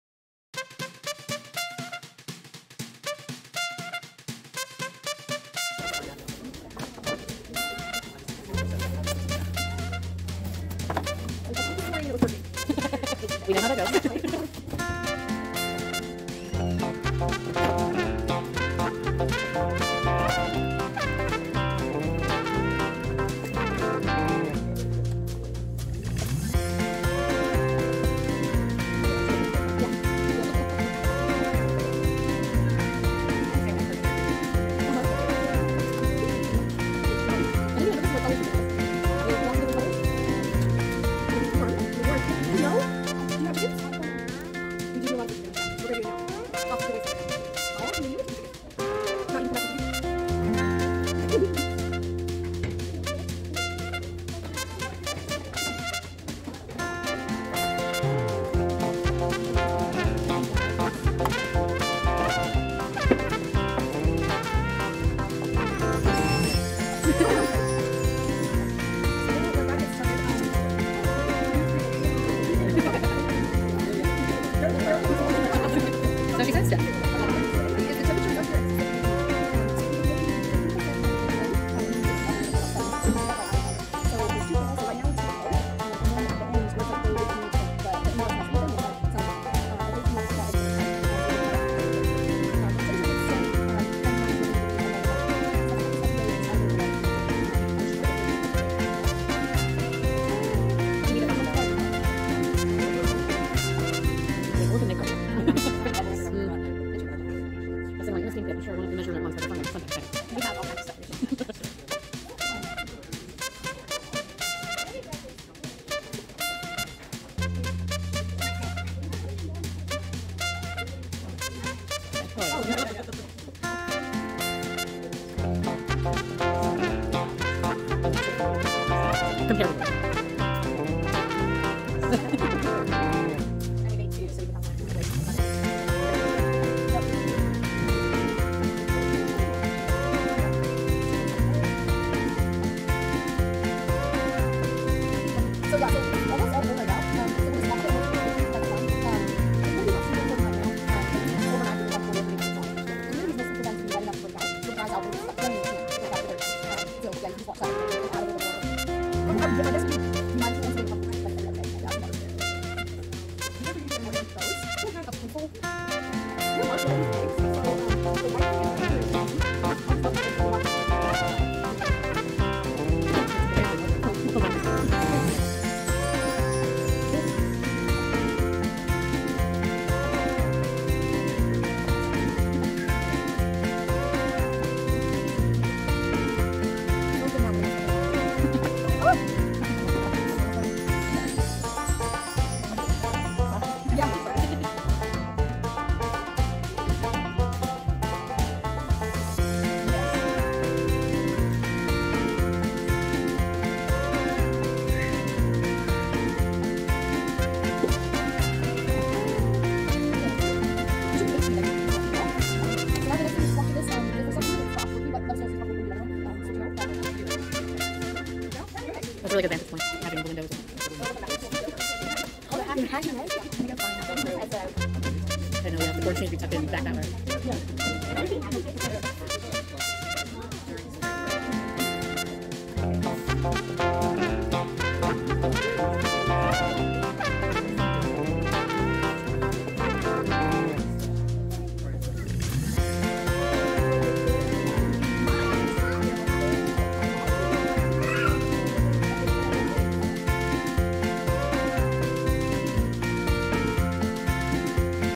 Eggstravaganza: Egg Incubator Setup at Davis Library
The chicken eggs are in the incubator at Davis Library!

We are partnering with Texas A&M AgriLife Extension to show the community the process of egg to chick, and you can see these chicks over the next few weeks.

Hatch Day will be a celebration of the baby chicks hatching, and all ages are welcome to join us on Tuesday, March 12 at 3pm at Davis Library.